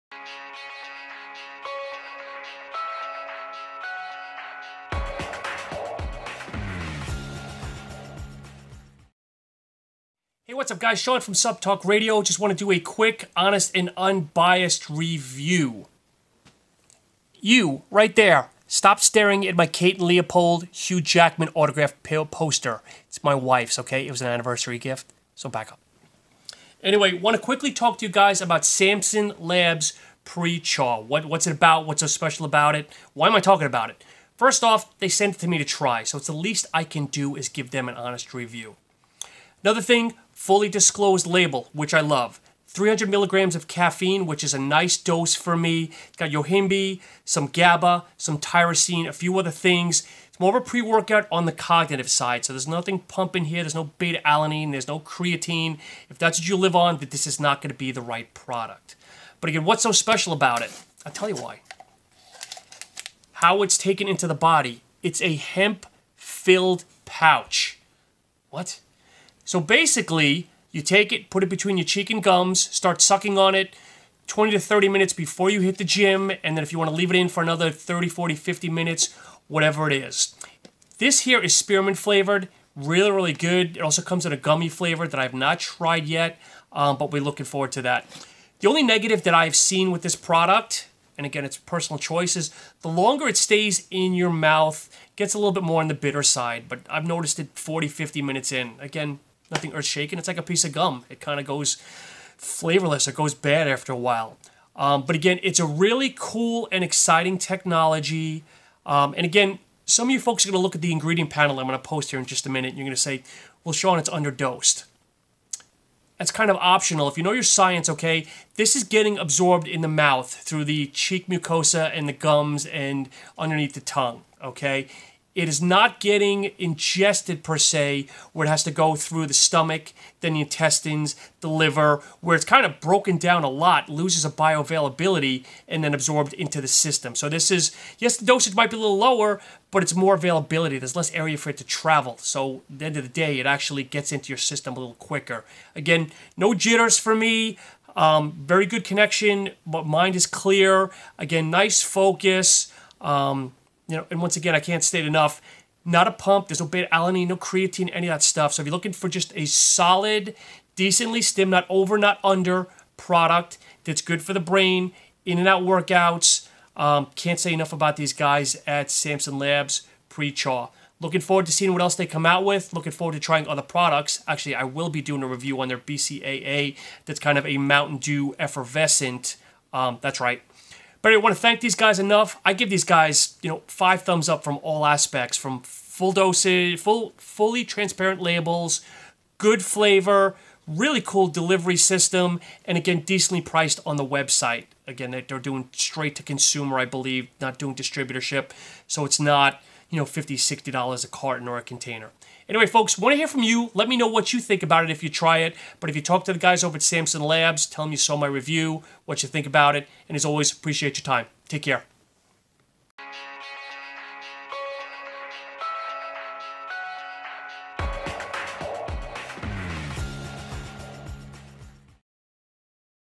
SuppTalk Samson Labs PreChaw Review
Quick and unbiased review if Samson Labs PreWorkout Pre Chaw

Full disclosure label, nicely stimmed, cool new delivery system